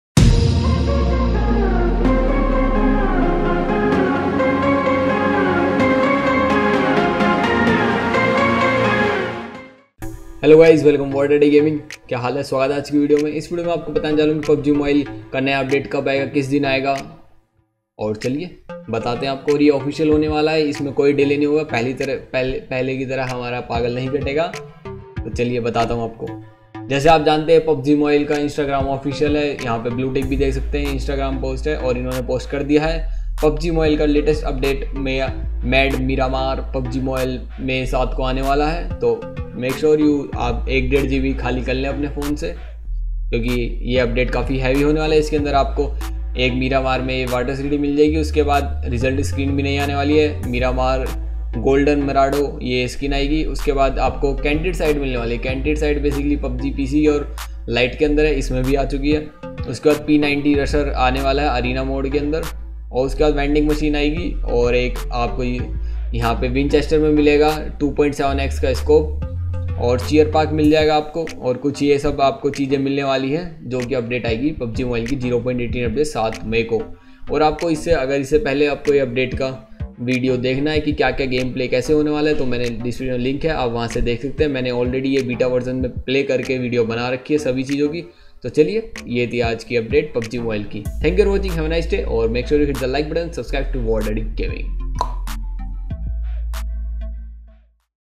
PUBG MOBILE 0.18 NEW UPDATE RELEASE DATE ANNOUNCED
it will releassd on 7 may 2020